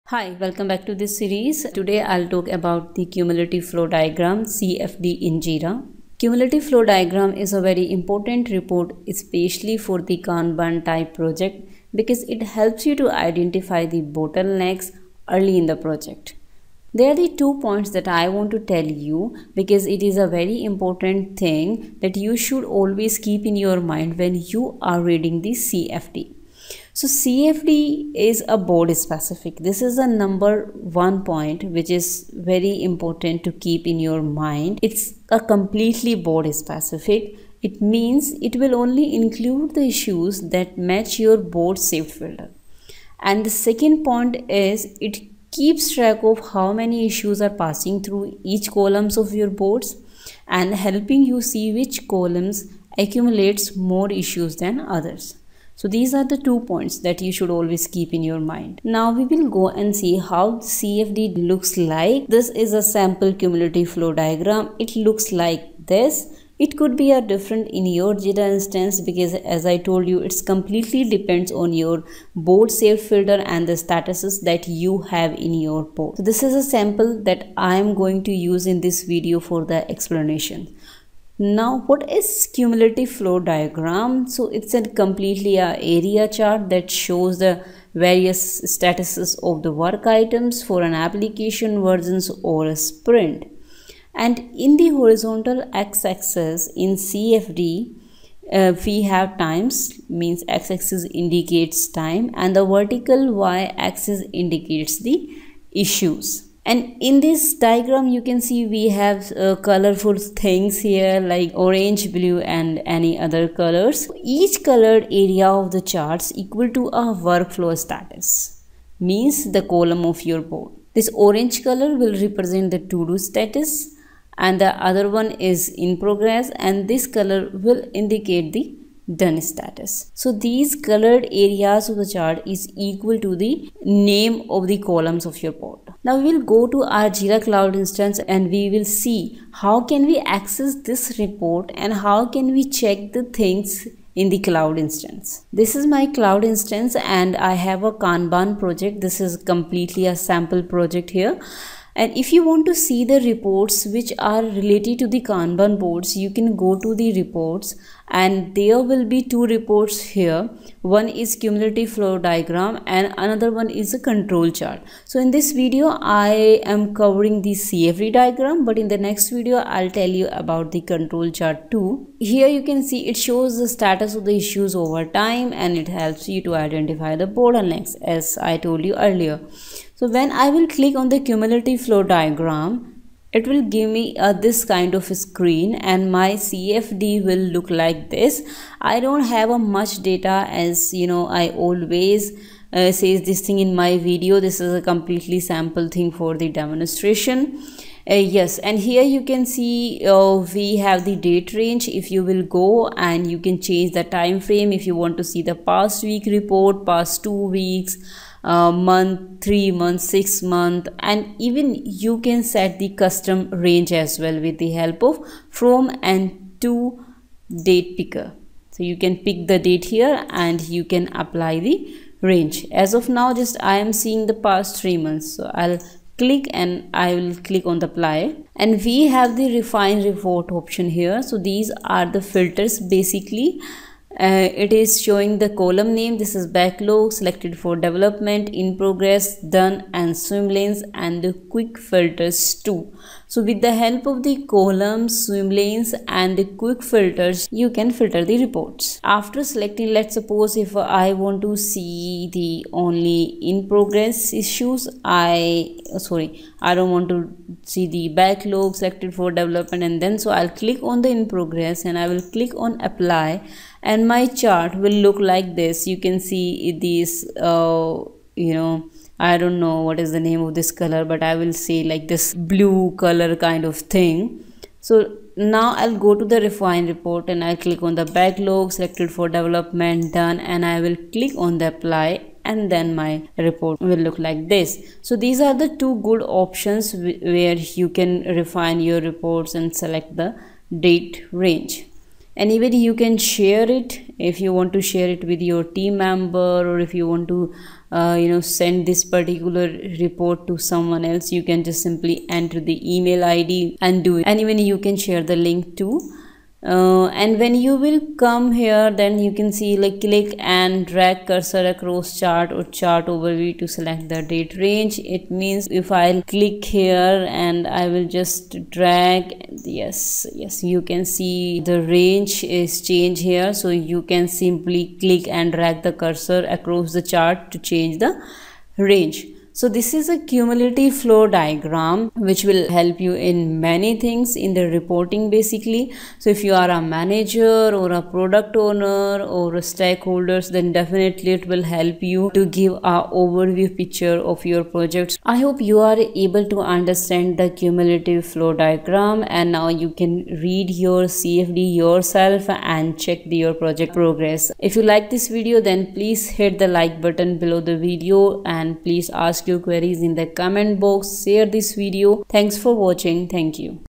Cumulative Flow Diagram (CFD) in Jira | Cumulative Flow Diagram kanban | Jira Tutorial
Cumulative Flow Diagram (CFD) : In this video, You will learn about the Cumulative Flow Diagram (CFD) in Jira. Cumulative Flow diagram is an area chart that shows the various status of work items for an application, version, or sprint. Cumulative flow diagram is for the kanban type project.